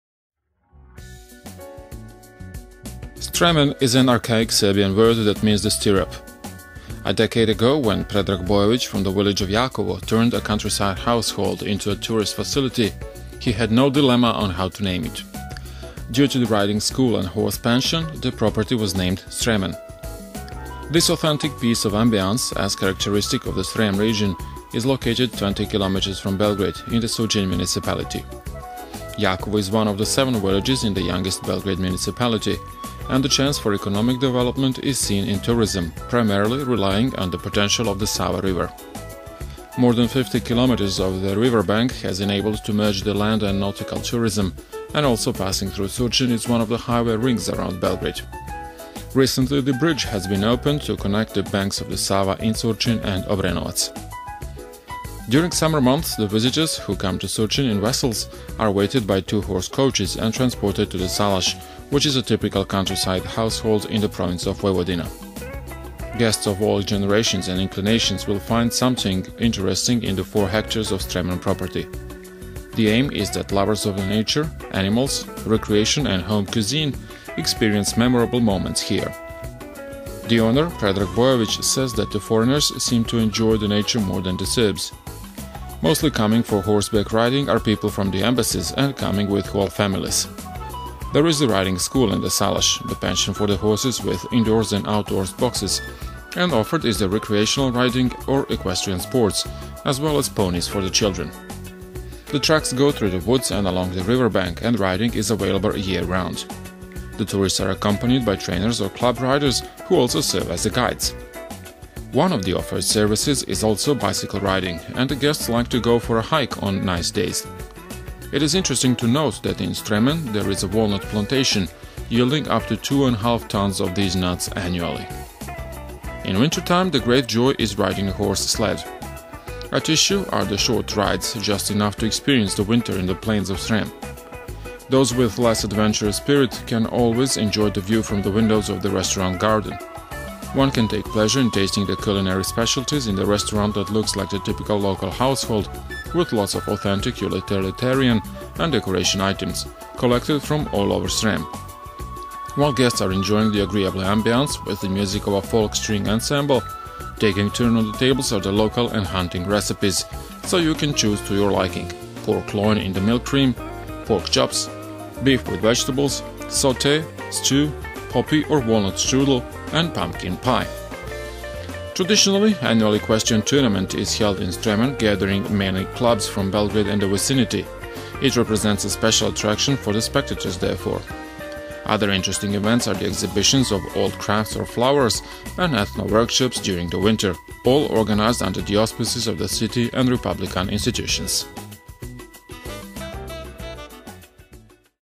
SALAS STREMEN
This authentic piece of ambiance, as characteristic of the Srem region, is located 20 km from Belgrade, in the Surcin municipality. Jakovo is one of the seven villages in the youngest Belgrade municipality, and the chance for economic development is seen in tourism, primarily relying on the potential of the Sava River.